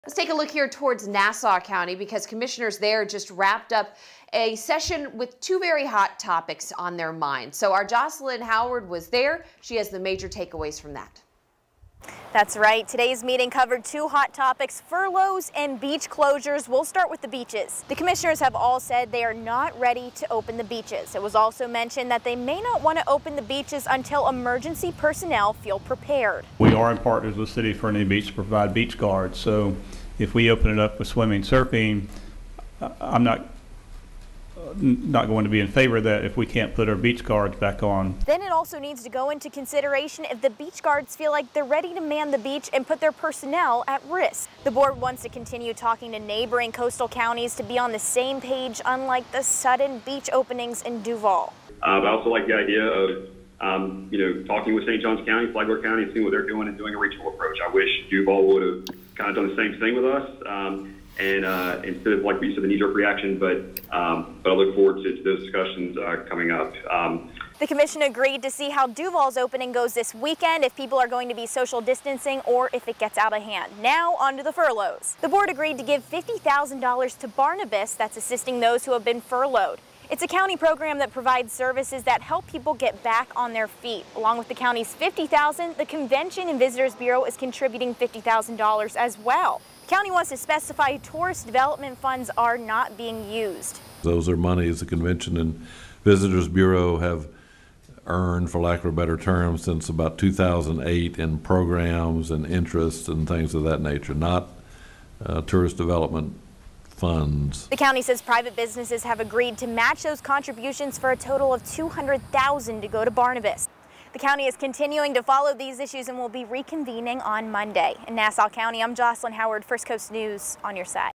Nassau County commissioners discuss furloughs, beaches
At this time, Nassau County has NOT made the decision to reopen its beaches amid the coronavirus pandemic.